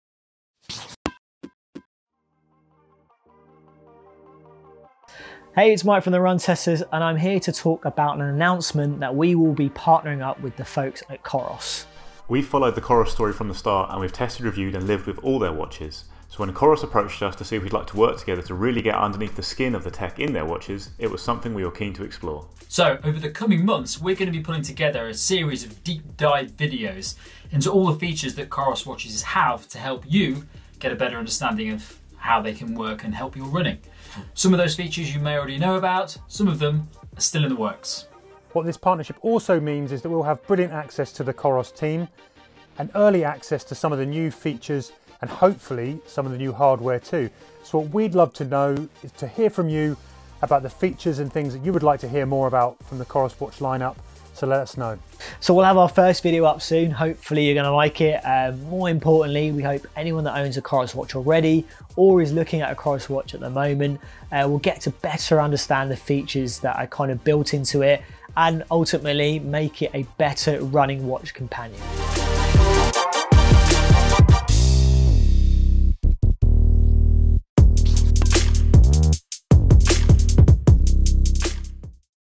The Run Testers Partners With COROS Wearables
For over a year The Run Testers have been testing the latest running shoes, headphones, watches and running kit spanning over 200 videos on YouTube. We've tested kit out over ultra-marathons, race-tested the latest carbon plate super shoes and compared the leading tech over many miles of training.

When wearable brand @COROSGlobal asked if we wanted to partner on a series of videos that dive deep into their range of watches, we jumped at the chance.

Over the next few months, we'll be releasing a series of collaborative videos that delve into the big COROS watch features aimed at runners, helping you to make the most of them.

We’ve been testing COROS products since the very first watch launched and we’re delighted to be working with them on this exciting video series.

The Run Testers is all about helping runners find the best products and over the course of this video series we want to help COROS watch owners new and old get the most out of their running companions.